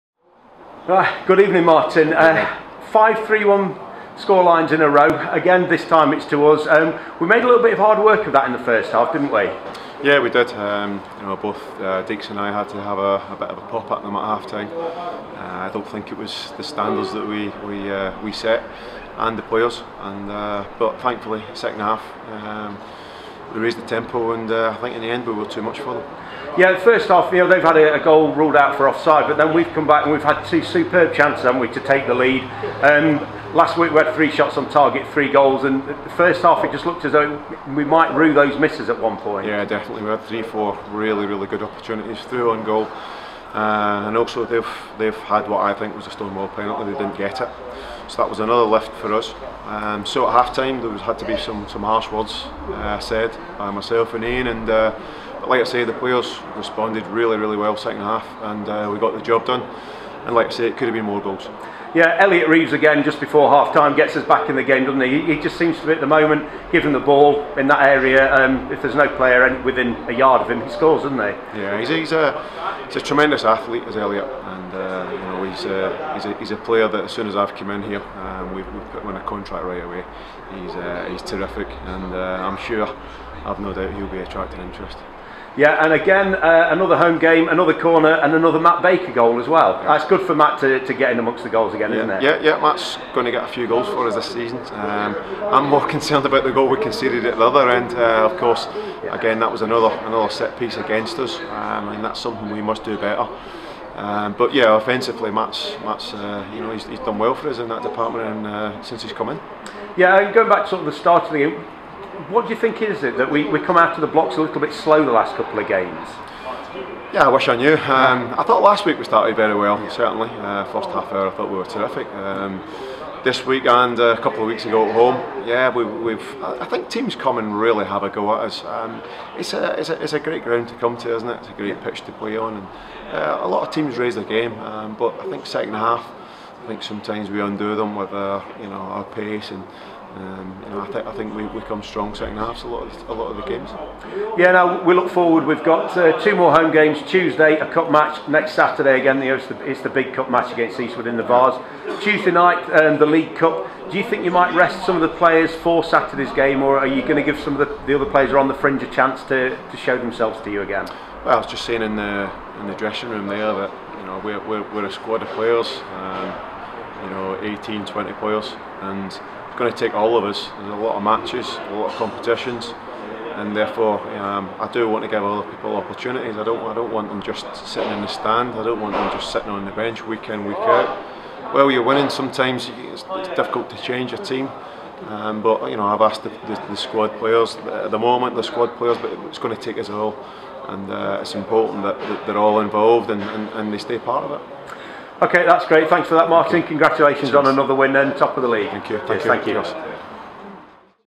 Martin McIntosh: Post Match Interview v Dunkirk (H)
Martin McIntosh Spoke to Craig Lamont, after seeing his side come from 0-1 down, to win 3-1 against Dunkirk FC.

⚽️ Next Home Game: Tuesday 27/11/18 v Brocton
🎟 Adults £5, Concessions £3, U16s Free